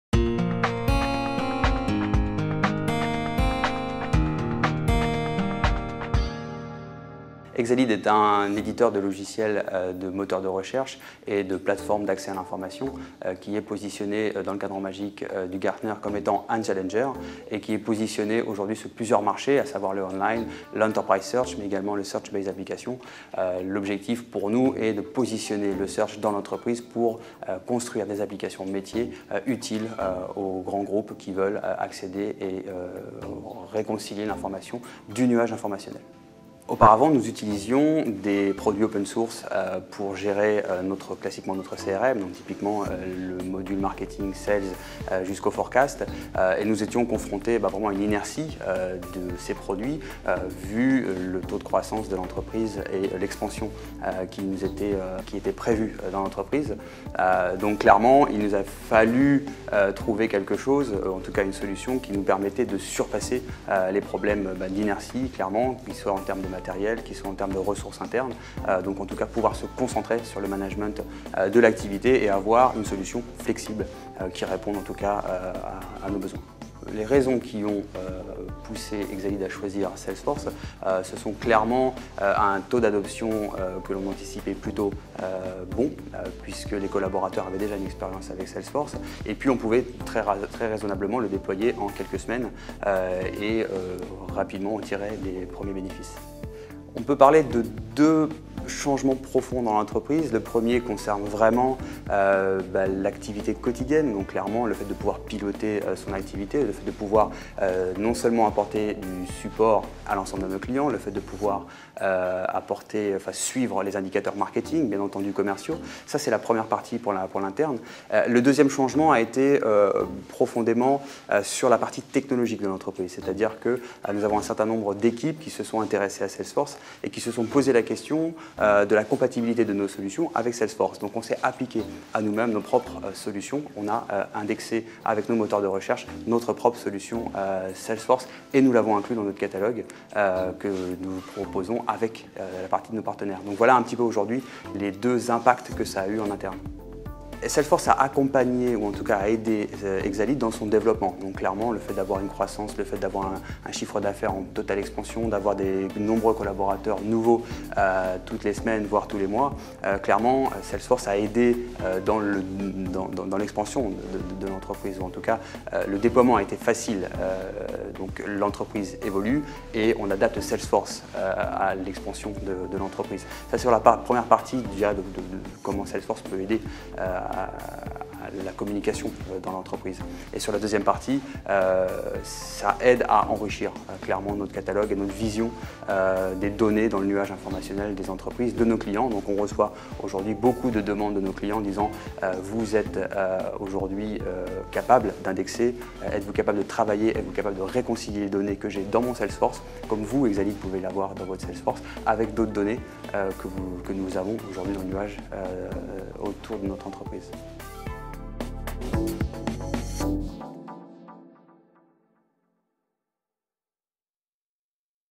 Exalead et le Sales Cloud : Salesforce.com
Salesforce accompagne Exalead dans sa croissance.  Grâce au Sales Cloud, Exalead pilote ses activités et son support client avec un fort taux d'adoption des utilisateurs